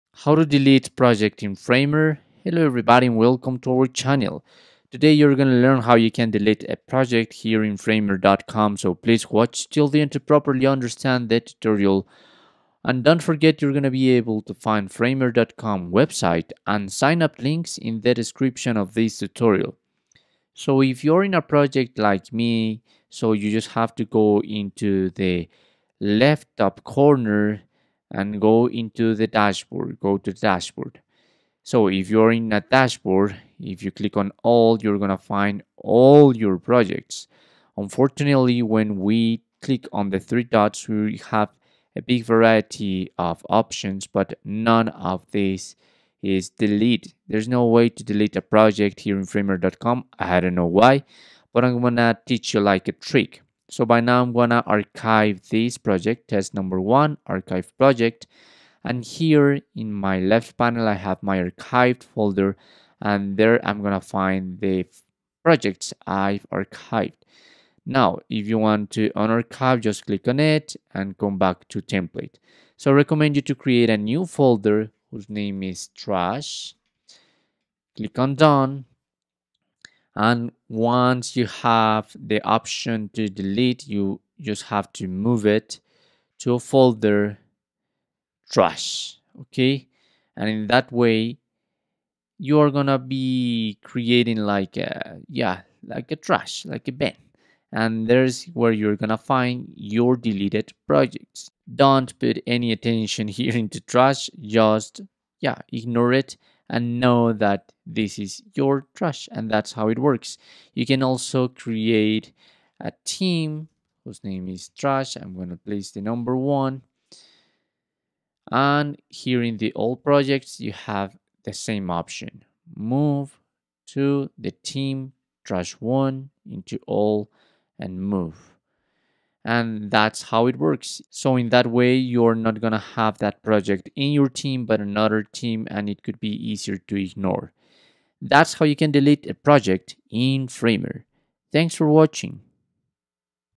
How to Delete Project in Framer (2026) Easy
In this video, we have shown How to Delete Project in Framer